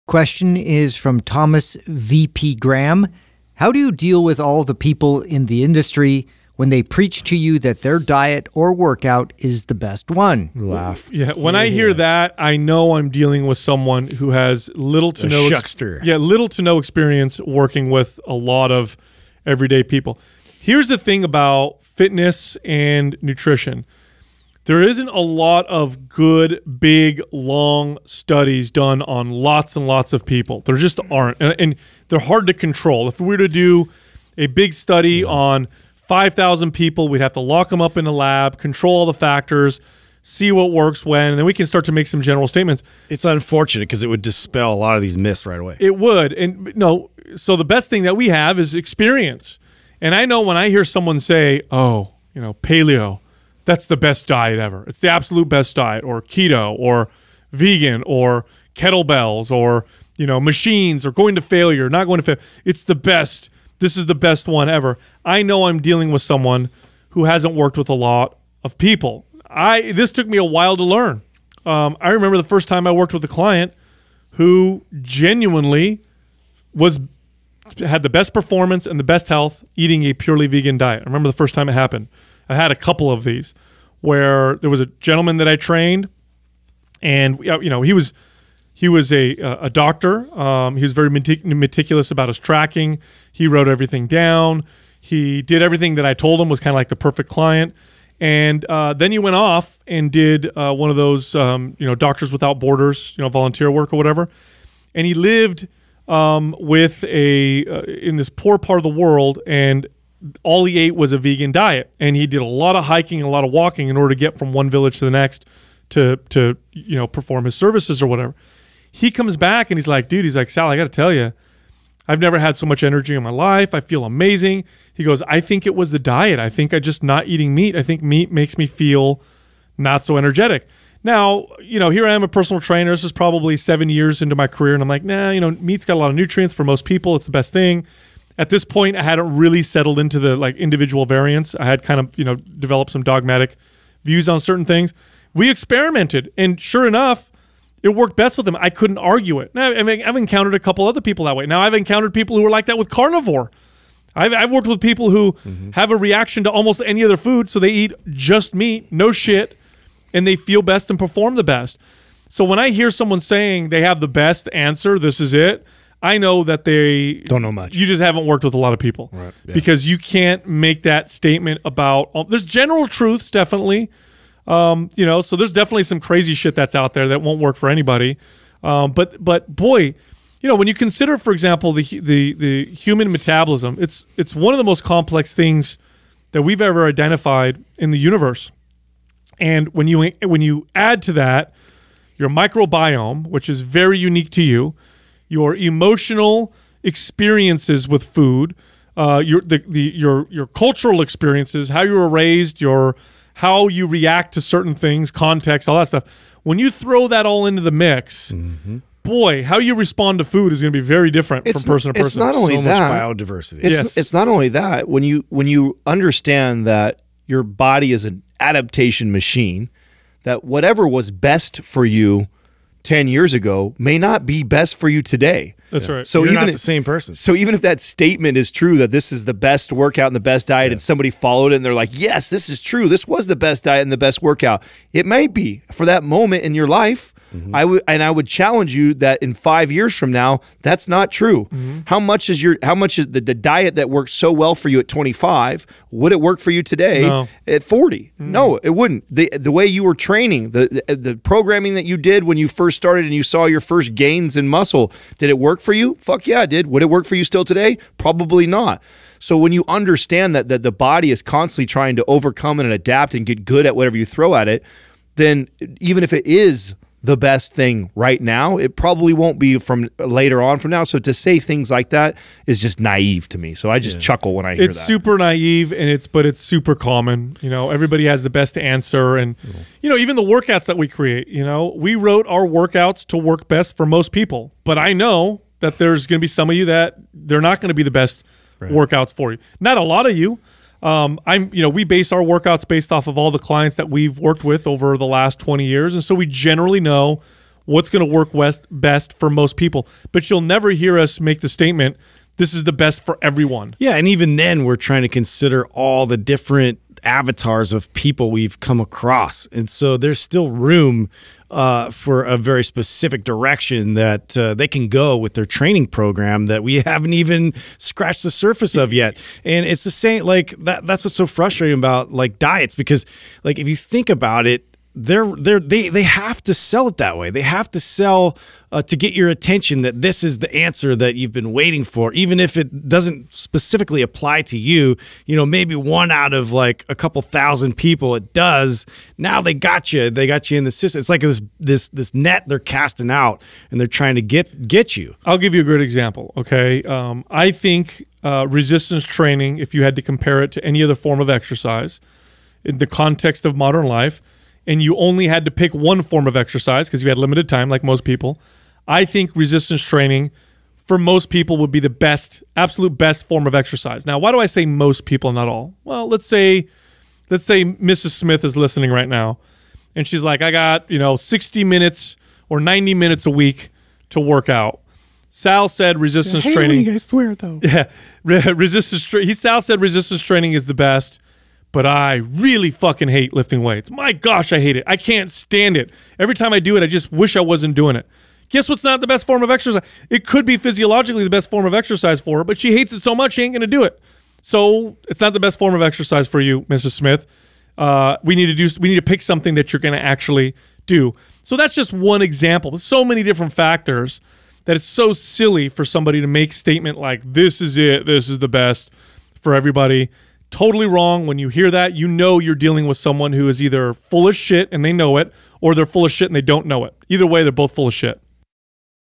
Dealing With Workout & Diet Zealots
In this QUAH Sal, Adam, & Justin answer the question "How do you deal with all the people in the industry when they preach to you that their diet or workout is the best one?".